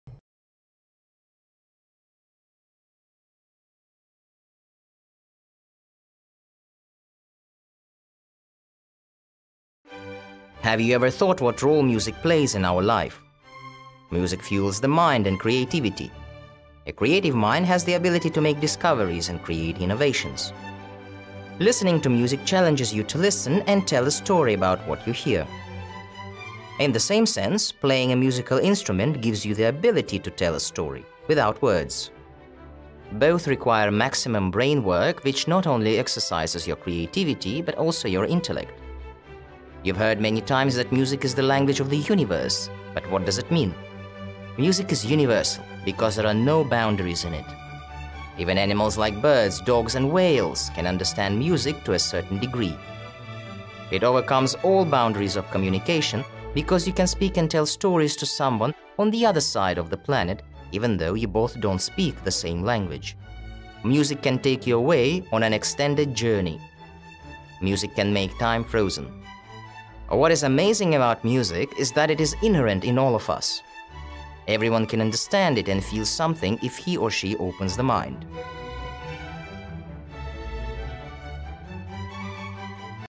Music
- Урок 27. Music
Автор: Макарова Юлия Игоревна

Речевой материал:

лексический: classical music, opera, jazz, folk, rock, heavy metal, pop, country, hip-hop, soul, reggae, musical instrument, challenge, overcome boundaries, be inherent in, violin, trumpet, cello, clarinet, harp

грамматический: распознавание и употребление в речи Present Simple, Present Continuous, Present Perfect, Present Perfect Continuous.